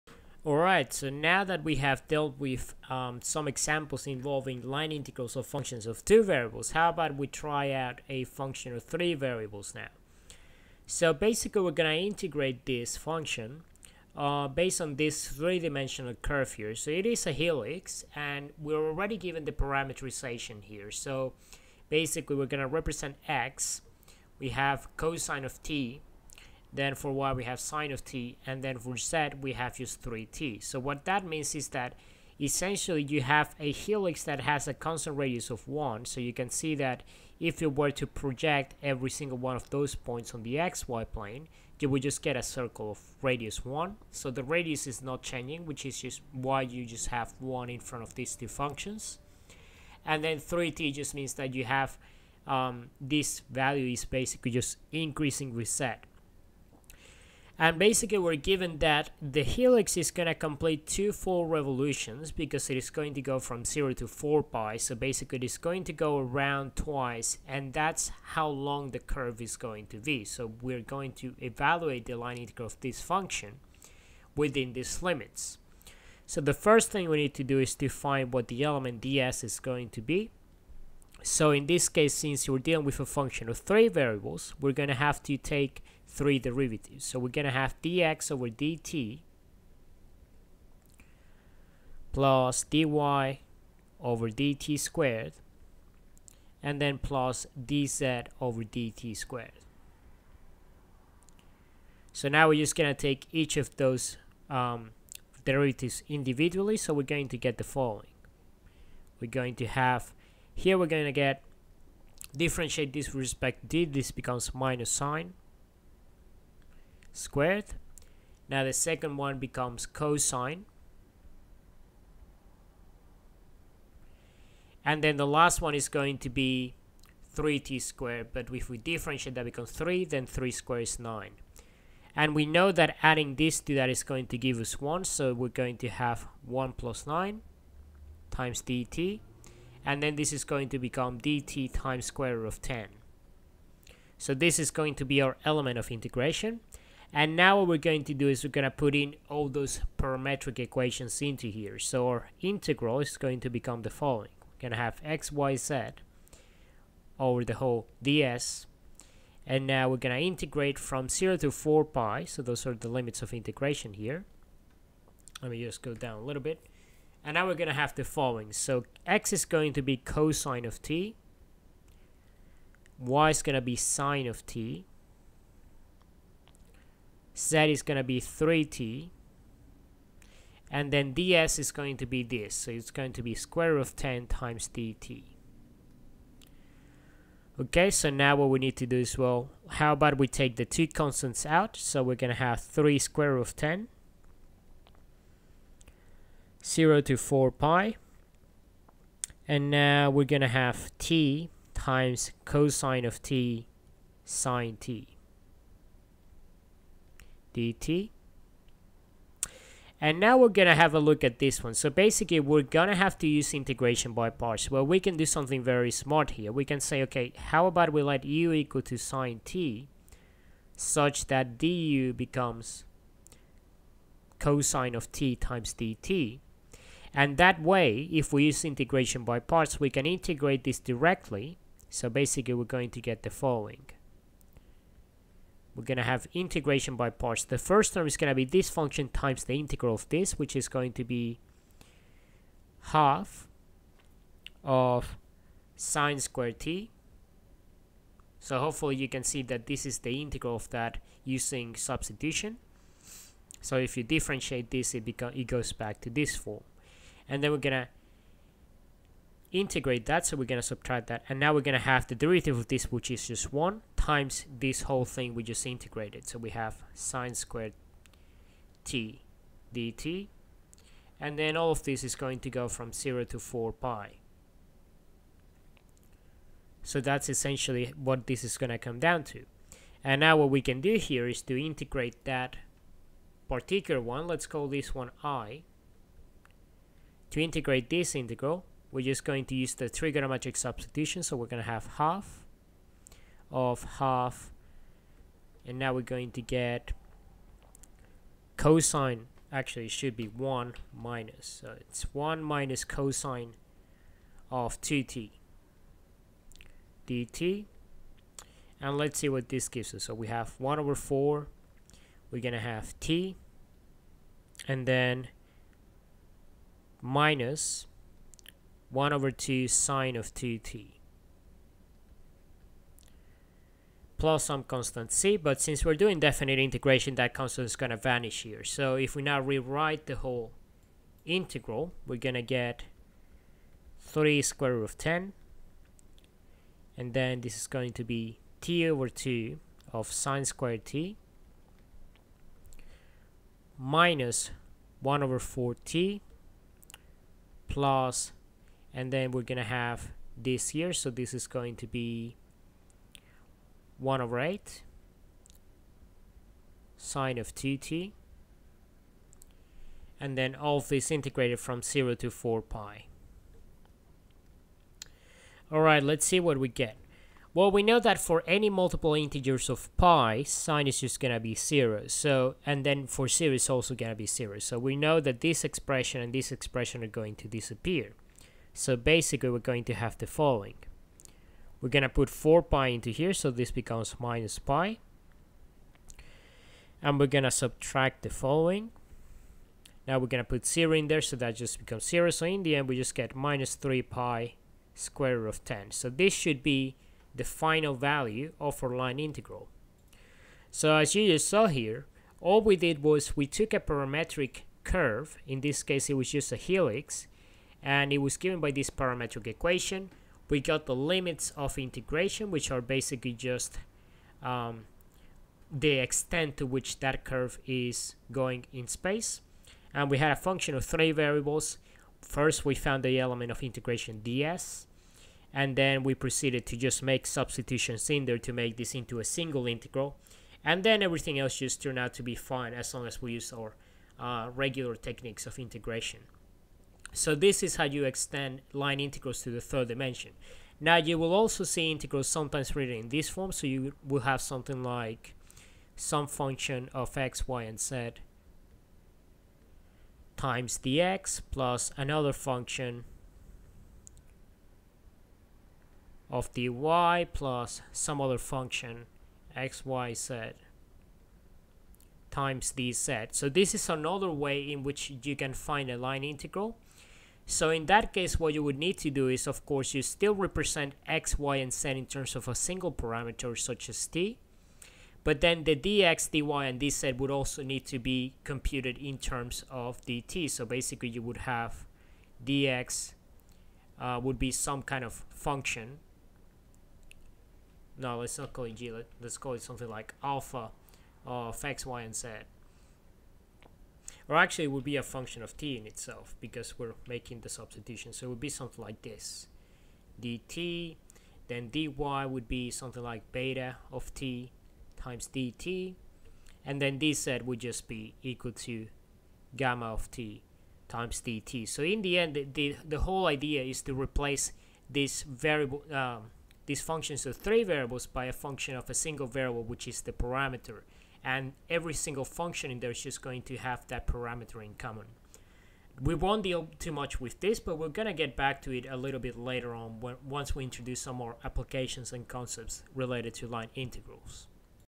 Line integral in 3D | Vector Calculus | LetThereBeMath
In this video we solve a line integral for a function f(x,y,z) over a helix.